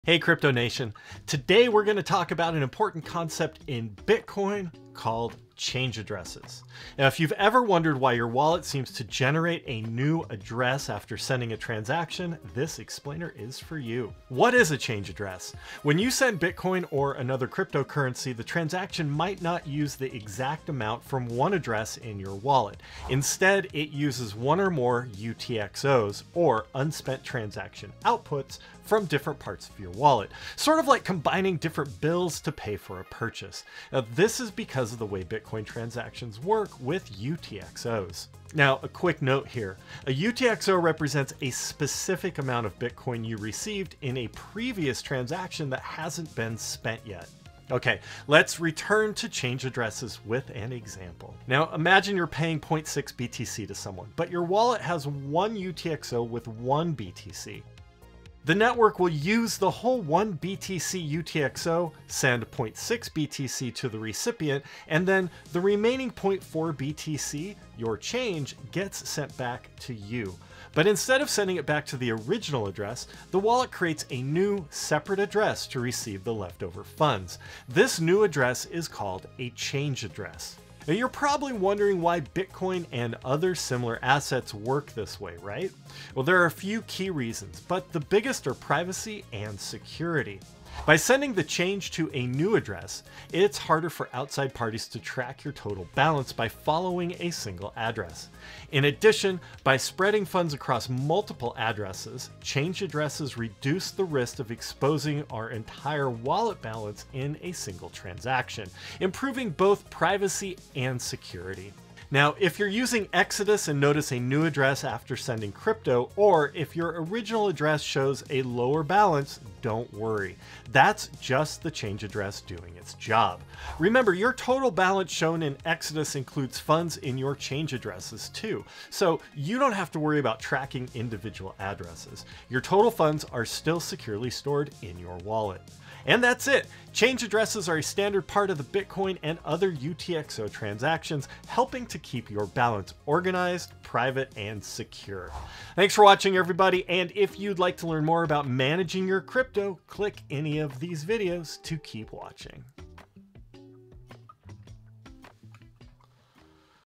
The Hidden Magic Behind Bitcoin Change Addresses EXPLAINED!
Ever noticed your Bitcoin wallet generating new addresses after sending a transaction? That’s the work of change addresses—a key feature of how Bitcoin and other UTXO-based assets manage your funds.

📘 In this video, we explain:

What are change addresses and why they matter

How UTXOs (Unspent Transaction Outputs) work in Bitcoin

The logic behind using new addresses for “change”

The benefits for privacy and security

What to expect when using Exodus or any wallet with UTXO-based coins

🚀 Whether you're wondering how to change crypto addresses, confused about your wallet showing a new one, or just want to understand the Bitcoin change address system, this guide breaks it down in simple terms.

💡 Quick Takeaways:

Change addresses help protect your privacy and wallet balance

They’re automatically managed by your wallet—no manual steps needed

Your total balance still includes any funds in change addresses

This is standard behavior in Bitcoin, Litecoin, and other UTXO networks

You should consult a qualified licensed advisor before engaging in any transaction. Exodus cannot guarantee the performance of the third-party platforms shown in this video. Its products/services, the steps shown, and the information provided may not always be accurate and are subject to change.

00:00 Introduction
00:11 What is a Change Address
00:23 What is a UTXO
00:38 How UTXO's work
00:51 Example of how Change Addresses work
01:23 Why Change Addresses Exist